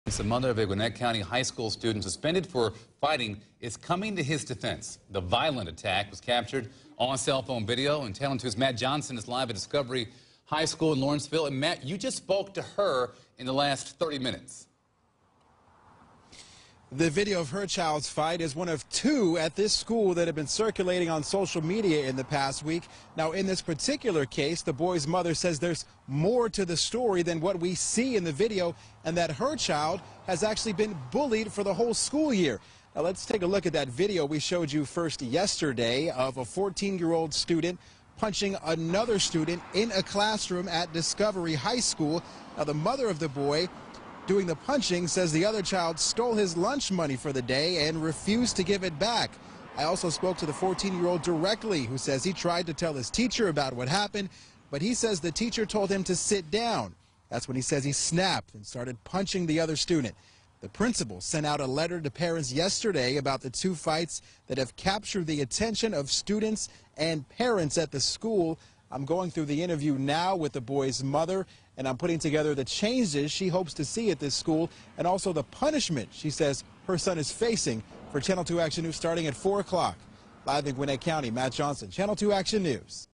Mother of teen in high school fight says her son was bullied
The mother says her son cracked after being bullied at Discovery High School in Gwinnett.